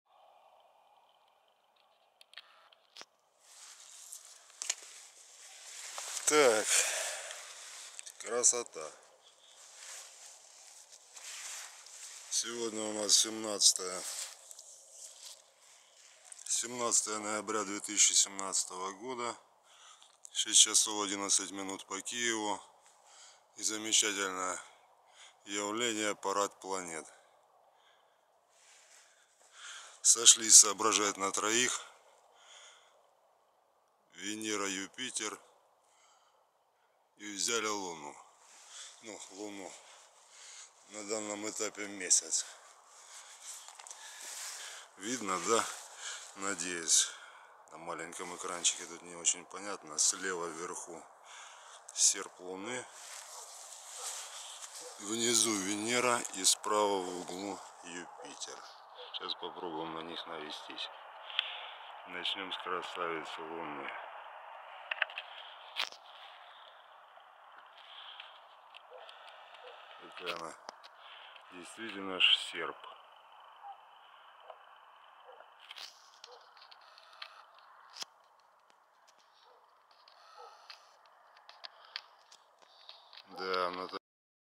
"Парад планет", треугольник Венера, Юпитер и Луна 17 ноября 2017, Харьков
Утром 17 ноября 2017 года Венера Юпитер и Луна образовали равносторонний треугольник - парадом планет это вряд ли можно назвать, в силу малого количества астрономических объектов что задействованы, но построением планет, небесных тел точно назвать можно.